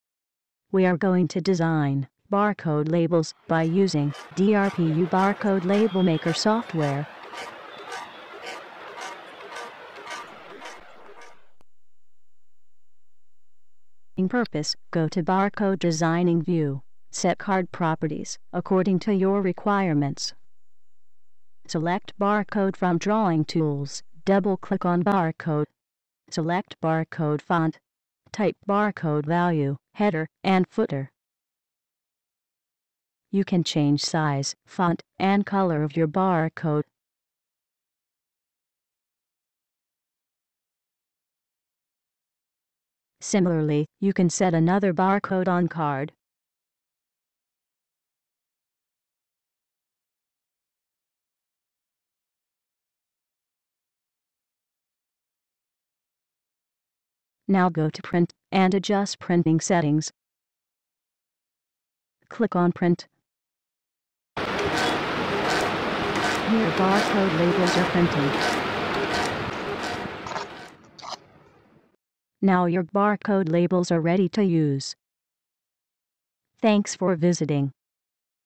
Linear Barcode Labels: Generate Barcode Labels in Linear Fonts
This video will illustrate you how to generate barcode labels in different linear font standards such as Codabar, Code 128, UPCA, LOGMARS and many others.